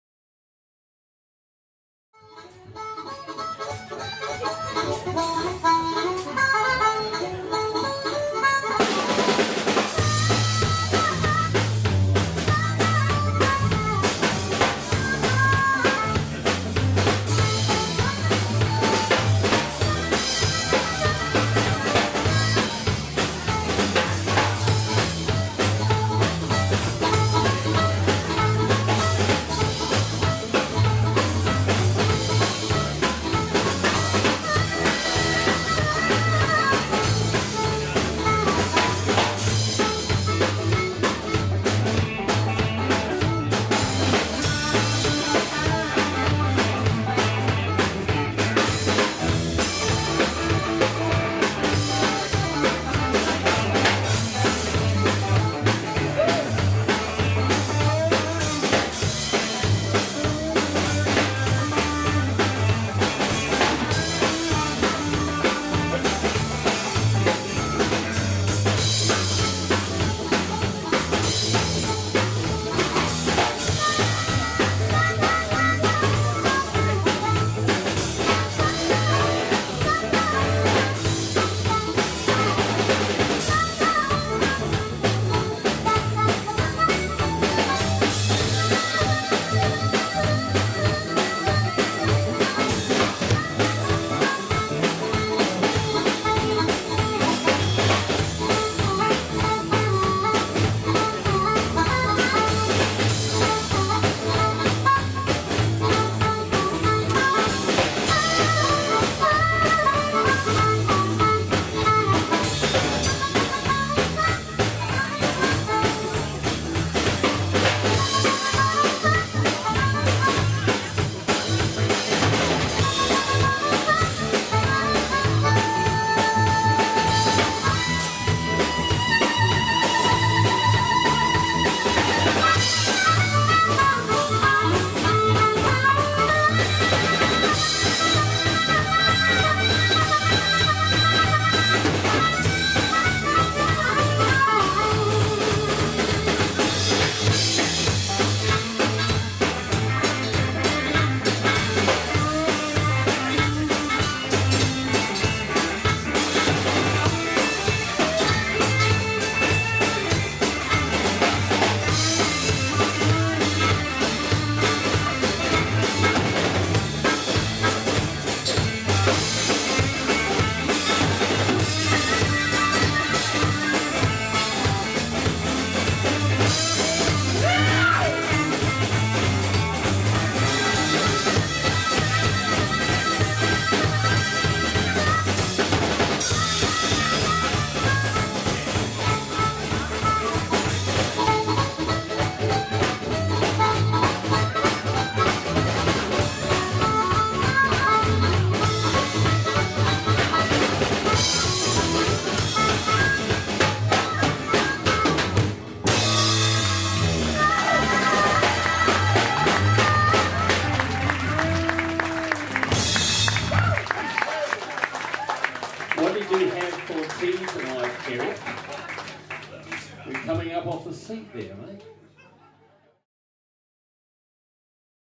11-09-2008 jam night - 12 bar jam
from the jam night on Thursday 11th September 2008, club president Nate on harmonica